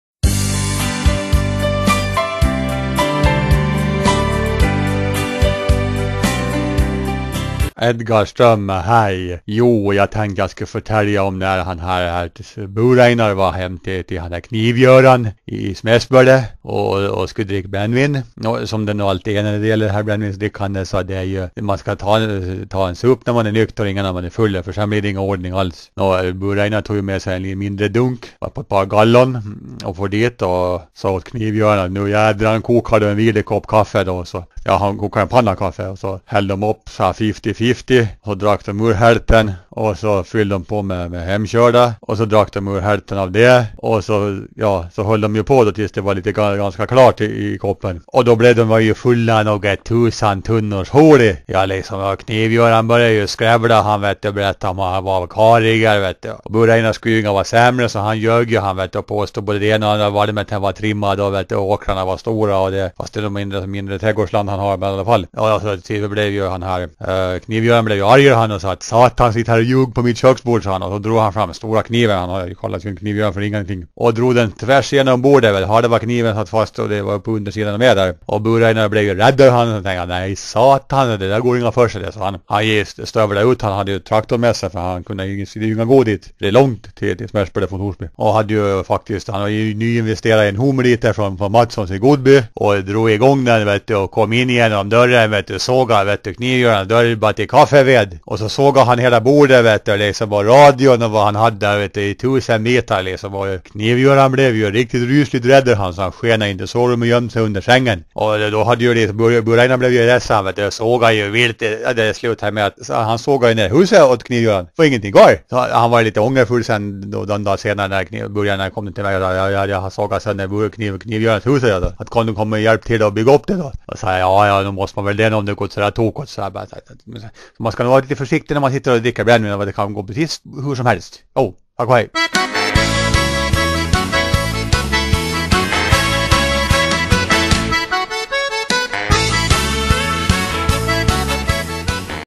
Edgar om nersågning.
Den fantastiske  Åländske bloggaren och kulturpersonligheten Edgar Ström berättar denna gång om när grannen Bur-Einar är på villovägar och får igång den nya motorsågen!